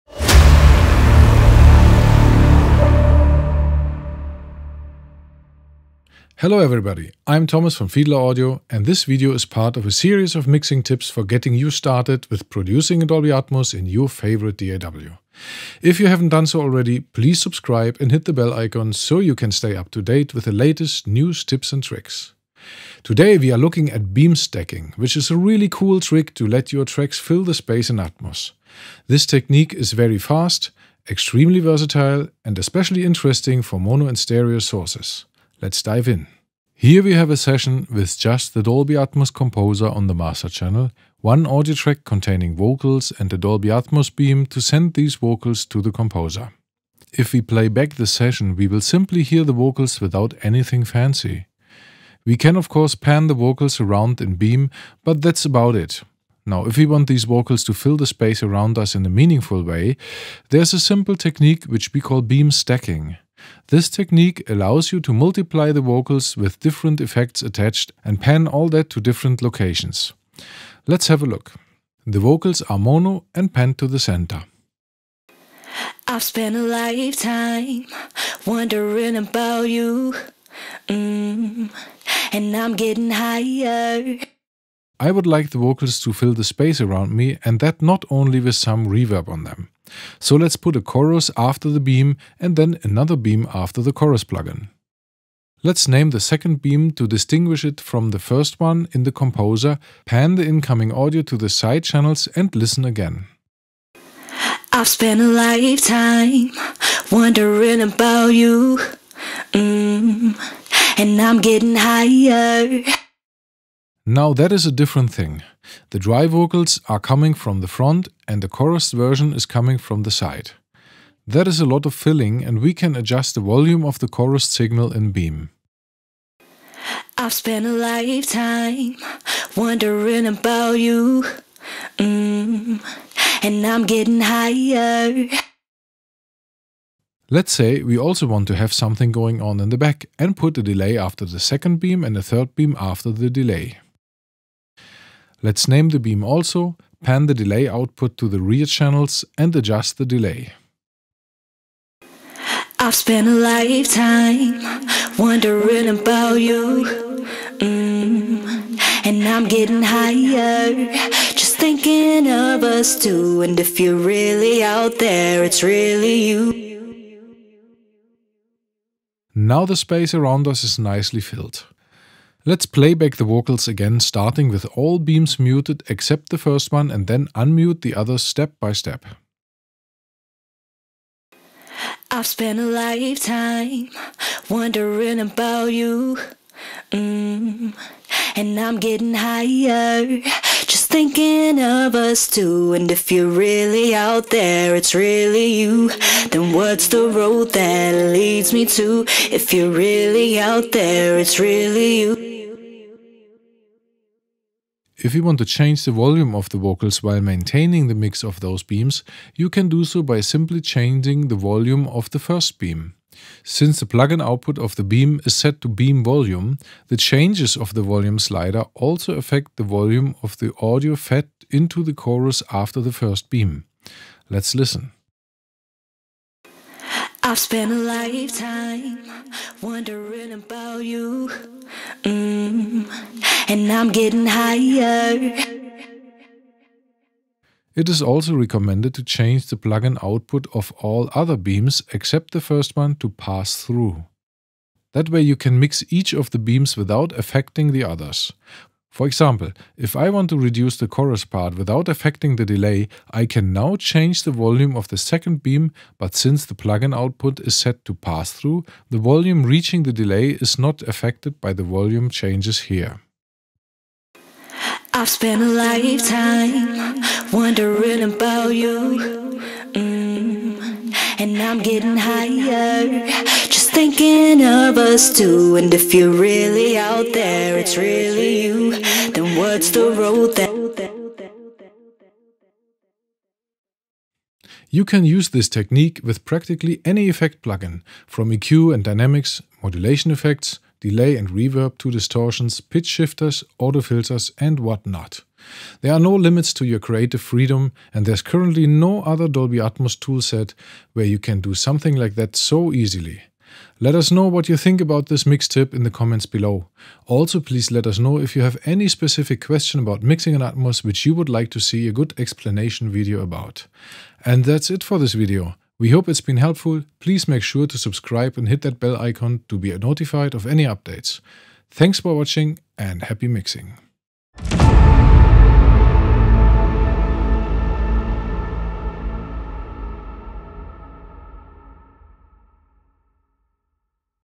Your tracks with FX in Atmos - Dolby Atmos Mixing Tip No.3
In this video we will get a first idea of Beam Stacking, a creative way to mix and spread your tracks with various effects into Dolby Atmos.

0:00 Intro
0:36 Step by Step Tutorial to Serial Beam Stacking
4:44 Outtro